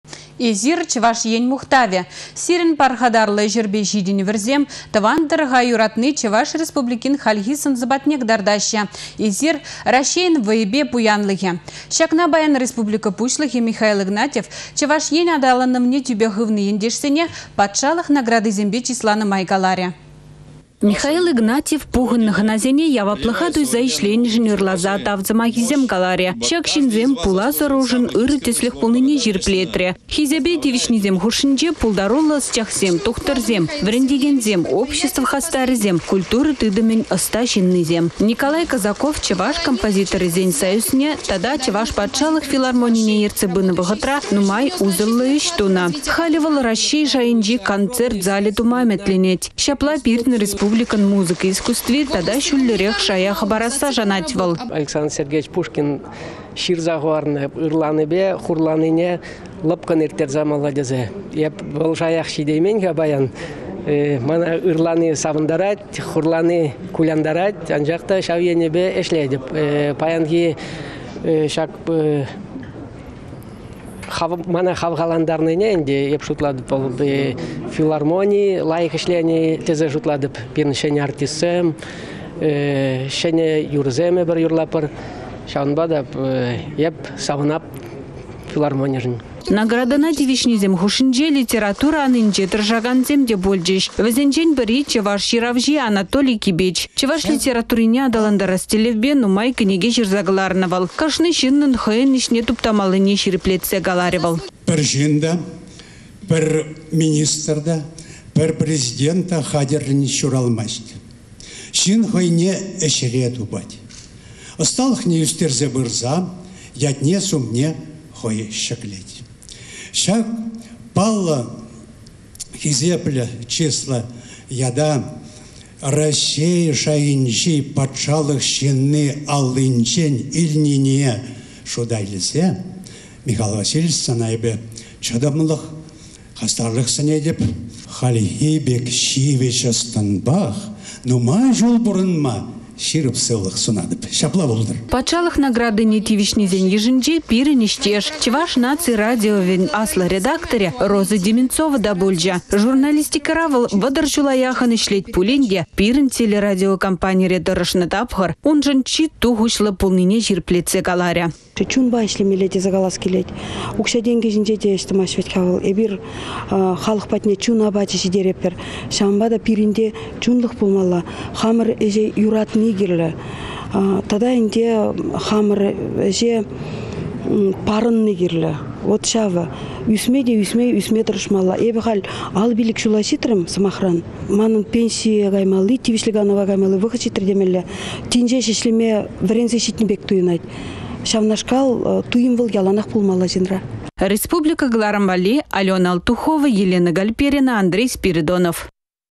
Ĕçре тăрăшуллисене патшалăх наградисемпе чысларĕç
«Эсир _ Чăваш Ен мухтавĕ! Сирĕн пархатарлă ĕçĕрпе çитĕнĕвĕрсем, тăван тăрăха юратни Чăваш Республикин хальхи сăн-сăпатне кăтартаççĕ. Эсир _ Раççейĕн вăйĕпе пуянлăхĕ!» Çакна паян республика  пуçлăхĕ Михаил Игнатьев Чăваш Ен аталанăвне тÿпе хывнă ентешсене патшалăх наградисемпе чысланă май каларĕ.